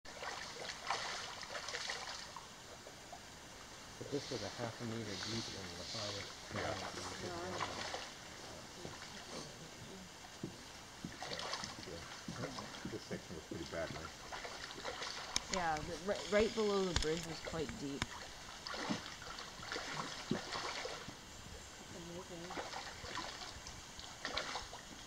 Cheakamus Centre Upper Paradise Channel Pink Salmon spawning
Pink salmon spawning in upper Paradise spawning Channel at Cheakamus Centre.